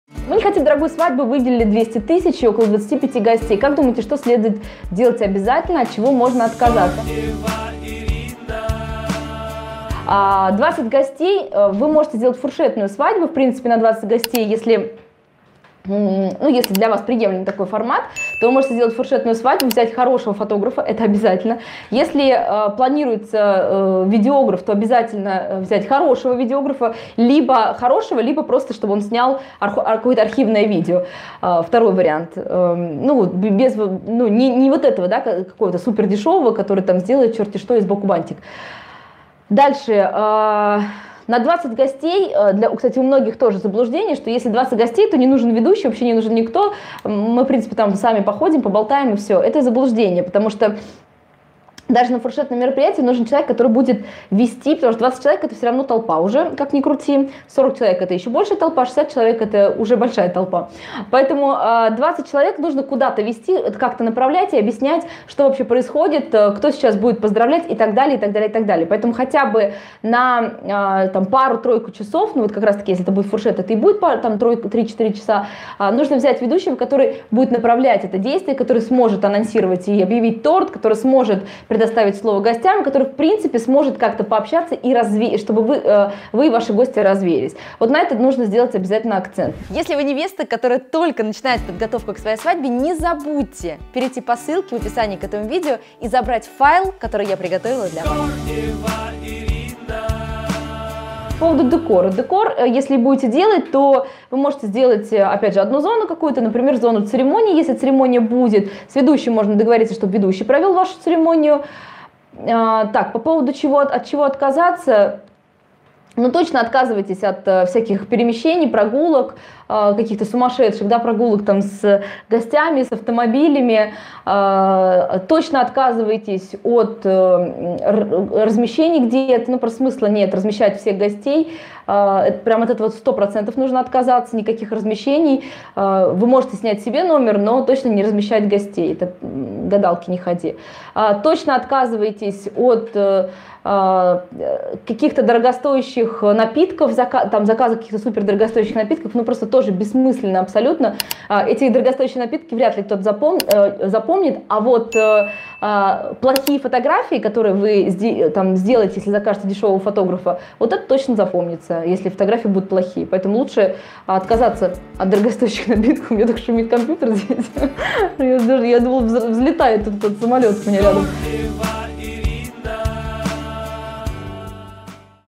Свадьба за 200 тысяч рублей. Чем жертвовать на бюджетной свадьбе?
Привет, мои невесты! Вот он вопрос невесты, которая понимает, что на бюджетной свадьбе не может быть всего. От чего-то придётся отказаться.
Итак свадьба за 200 тысяч рублей. Чем жертвовать на бюджетной свадьбе?
Первым делом надо понимать, что свадьба в Томске ха 200 тысяч рублей и свадьба в Москве за 200 тысяч рублей это совершенно разные свадьбы.
Во втором случае отказаться нужно будет от большего.
Отвечать в описании не буду подробно, ответ на вопрос от чего можно отказаться при подготовке к свадьбе, вы можете посмотреть в этом выпуске.